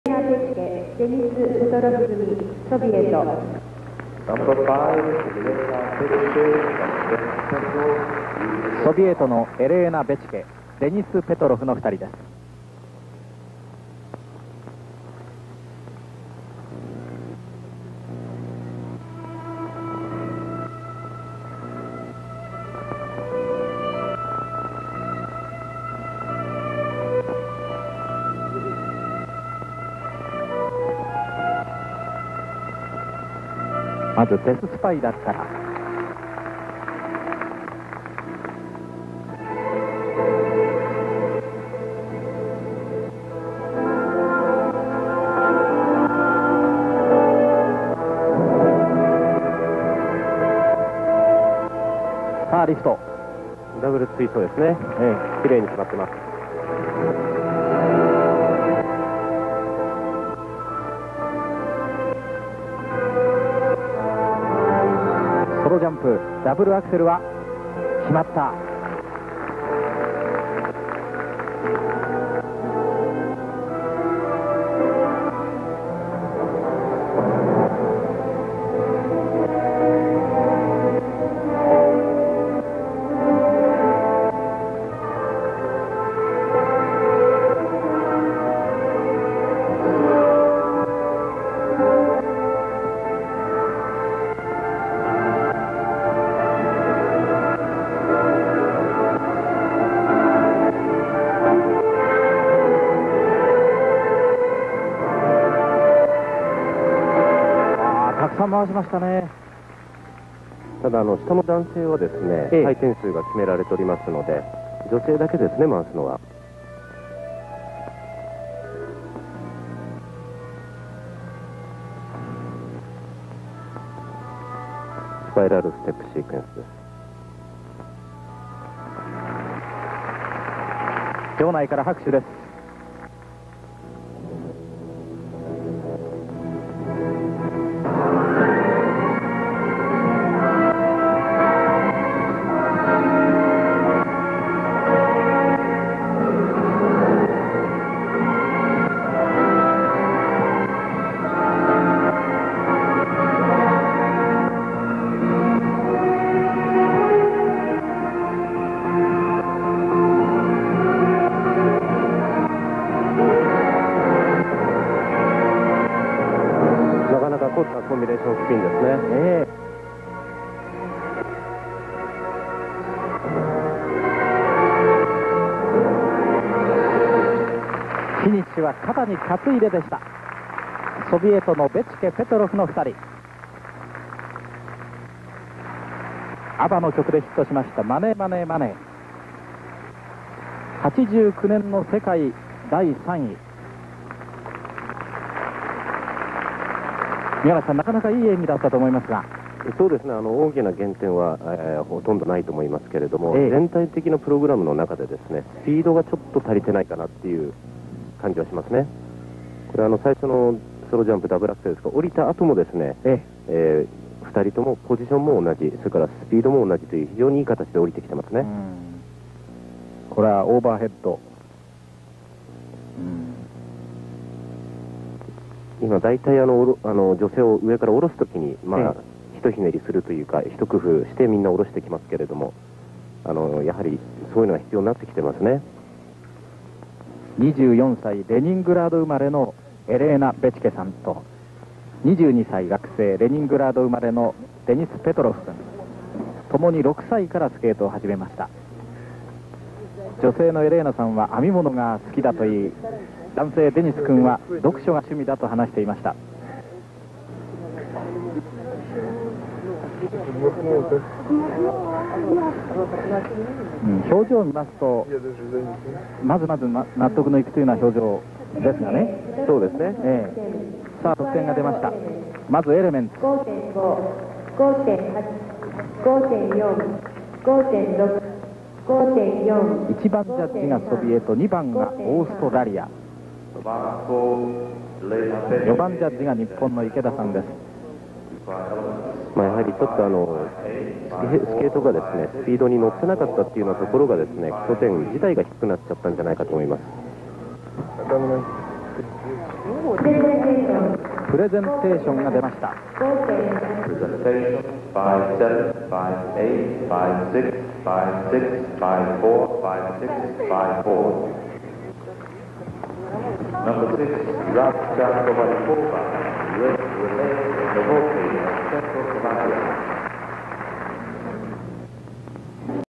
Elena Bechke and Denis Petrov: 1990 NHK Trophy: Short Program
Elena Bechke and Denis Petrov take first place in the short program at the 1990 NHK Trophy.